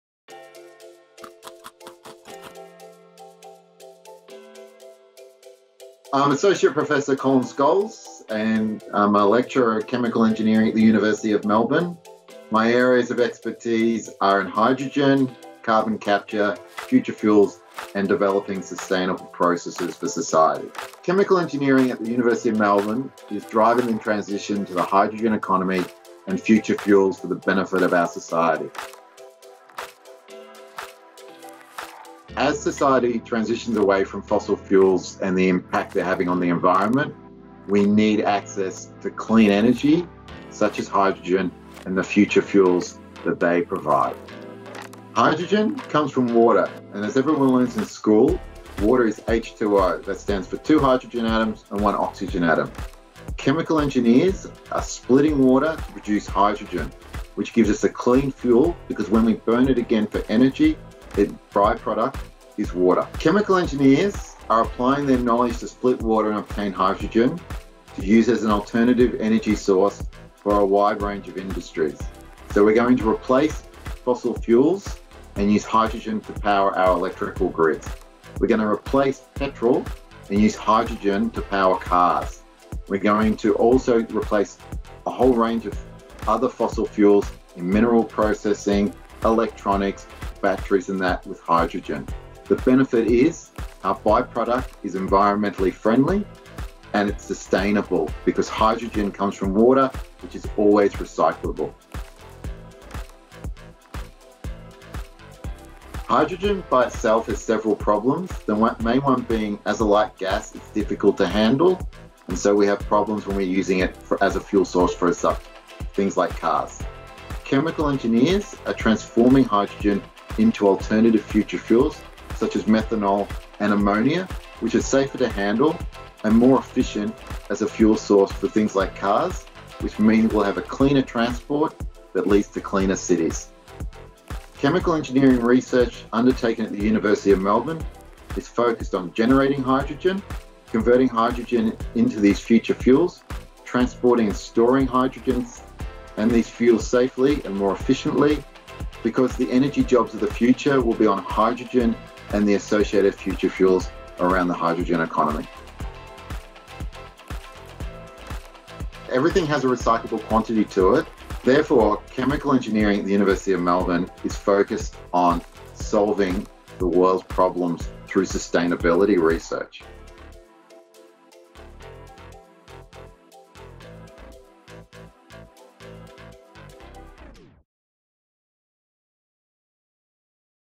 Bite-sized Lecture: Fuelling the future with hydrogen with Professor Colin Scholes
What role does hydrogen play in a more sustainable energy future? Professor Colin Scholes gives a quick explanation in this Bite-sized Lecture.